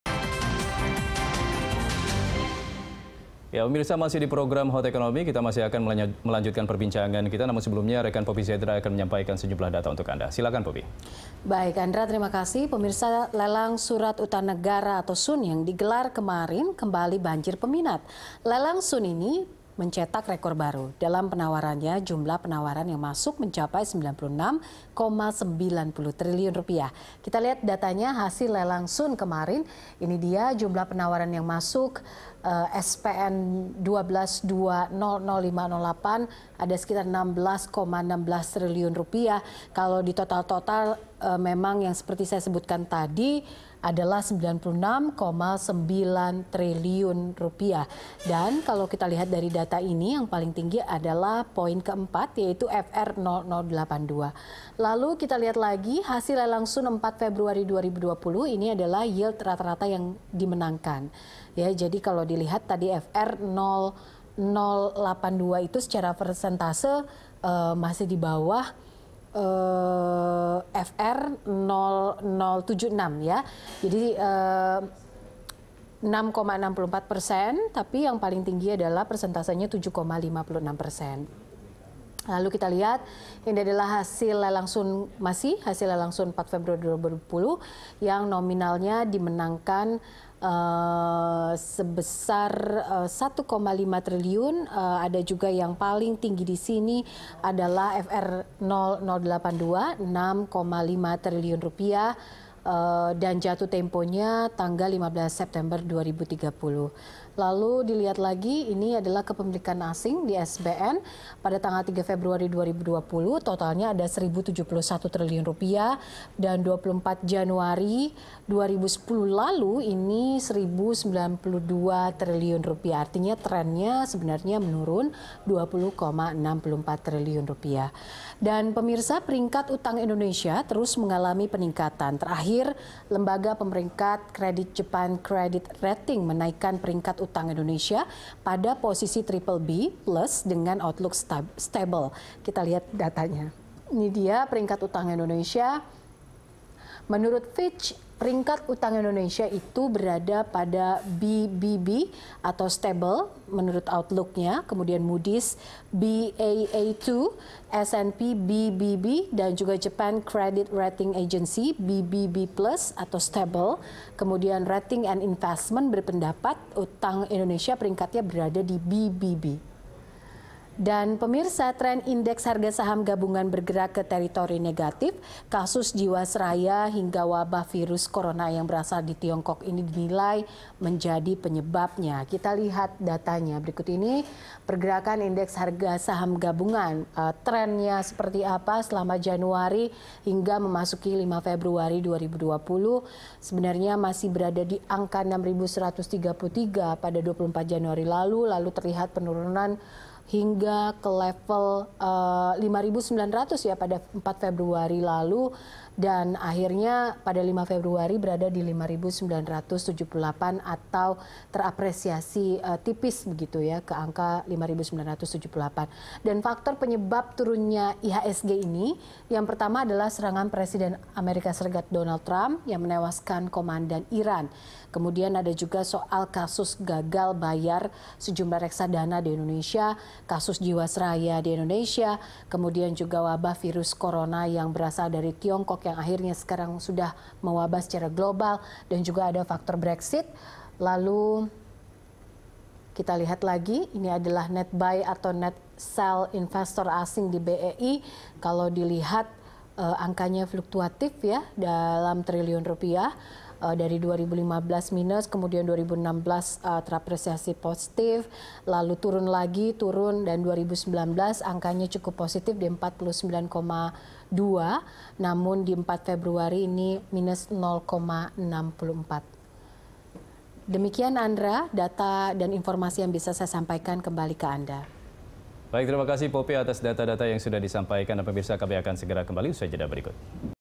Hot Economy: Korona Merebak, Sun Diserbu #2
Tak hanya menewaskan manusia, virus corona juga merontokkan harga saham, melumpuhkan pariwisata, dan menghambat kemajuan ekonomi. Tapi para pemodal asing justru menyerbu surat utang negara atau SUN.